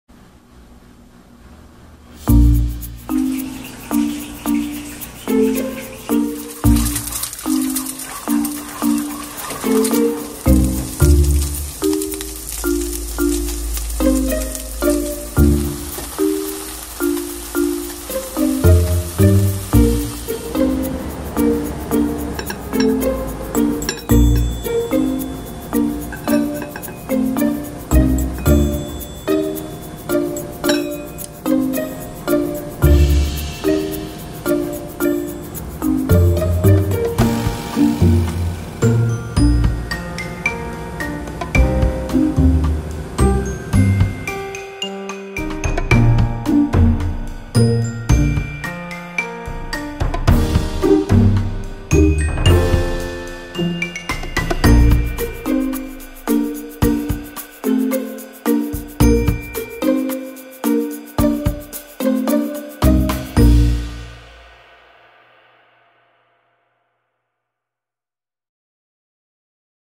[부산남매][스팸마요덮밥] Spammayo bowl of rice
재료 : 스팸, 계란, 양파, 밥, 마요네즈, 데리야끼소스, 김
1. 계란을 스크램블 한다.
2. 스팸을 굽는다.
3. 양파를 데리야끼 소스랑 볶는다.
4. 밥에 계란, 양파, 스팸을 넣고 김을 뿌리고 마요네즈를 뿌린다.